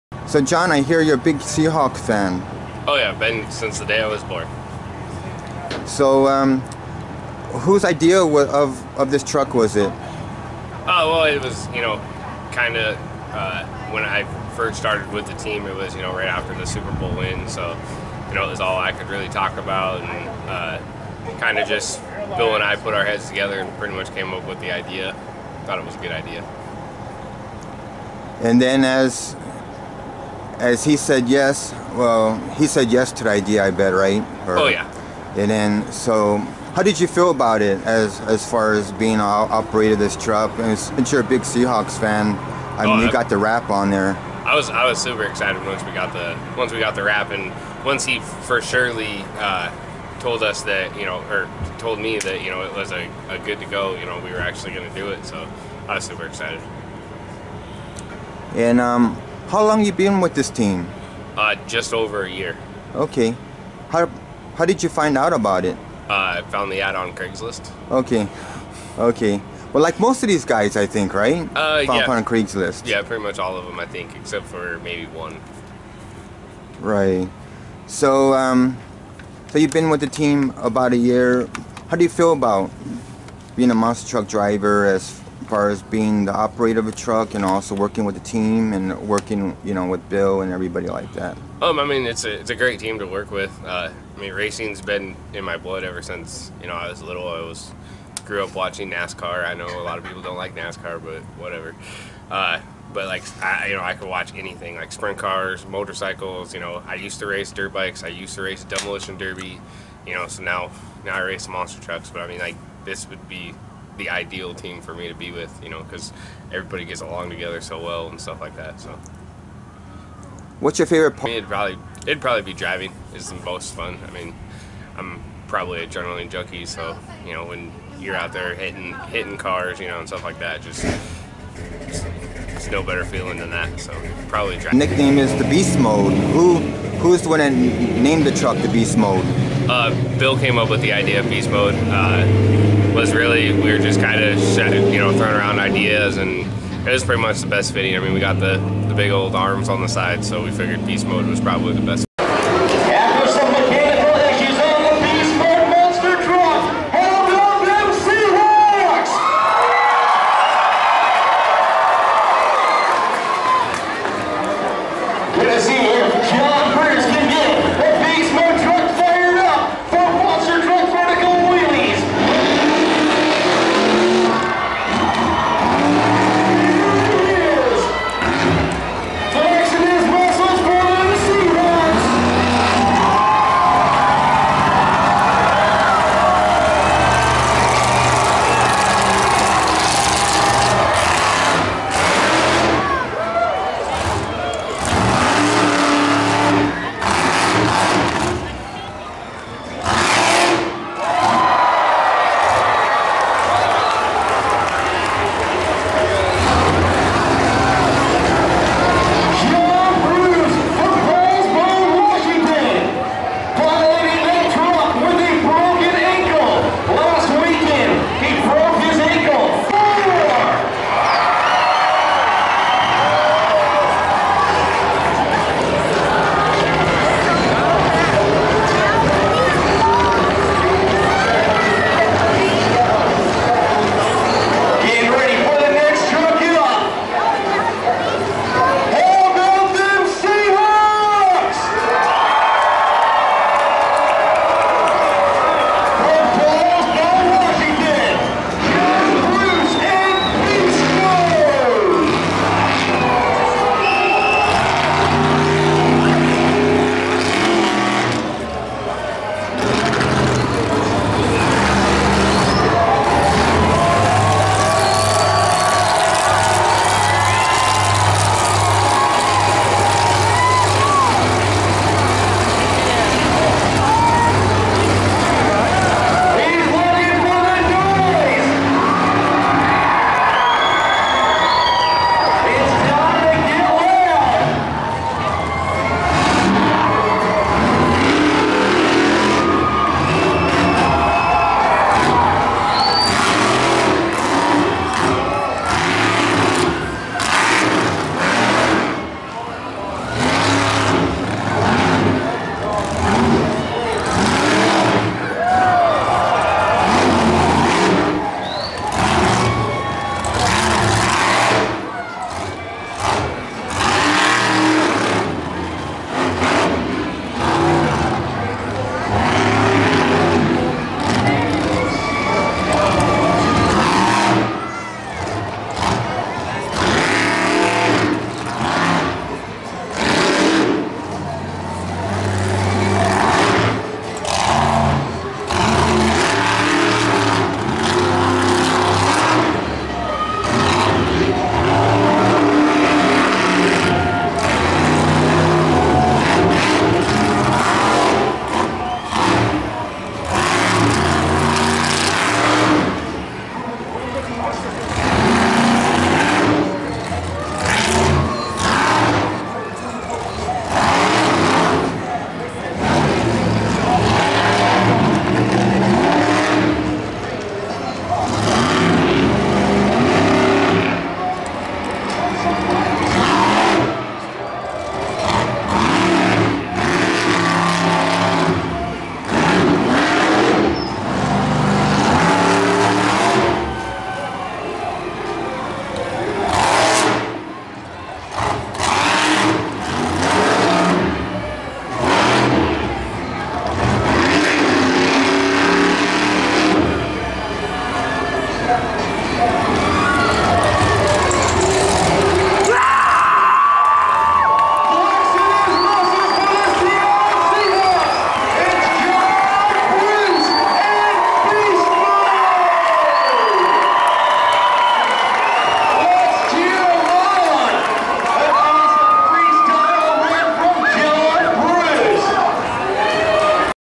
Straight Up Racing Beast Mode John Bruce interview-track @ Yakima 2016
May 21 2016 Yakima,Wa. Sat 7p.m. show Funtime promotions monster truck show.Seattle Seahawks monster truck.